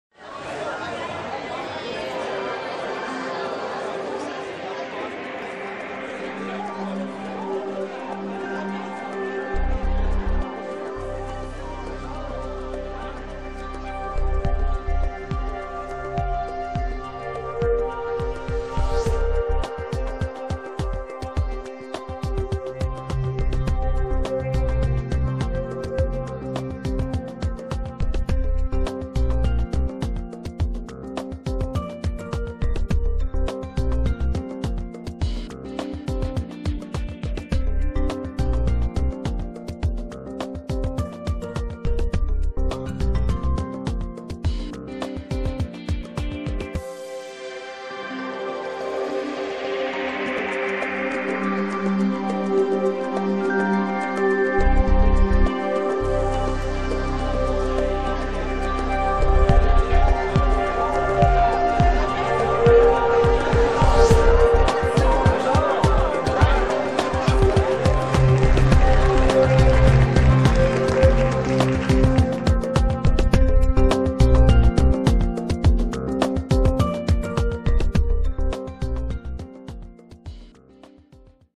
Painting Demonstration in Taos, New Mexico
A short video of a painiting demonstration in Taos, New Mexico.